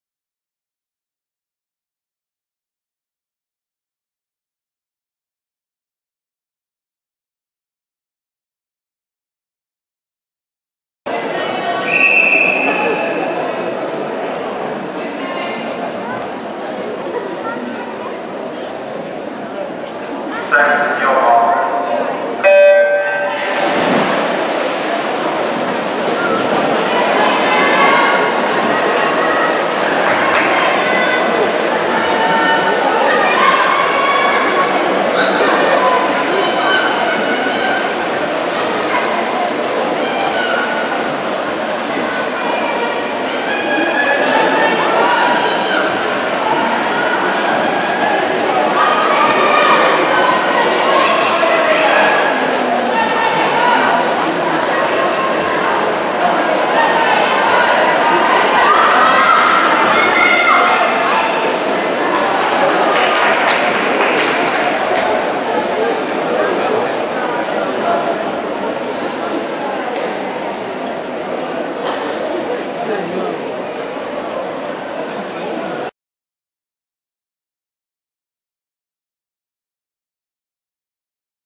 7 year old Monica Botha women 8 &U 50m freestyle 40:34
Monica Botha, aged 7, registered as a Level 1 swimmer in May 2014 after joining Seagulls swimming club.  This was her first 50m freestyle at her first age group gala women 8 & under.  The girls she competed against were all 8 years and older - most of them turn 9 late in 2014.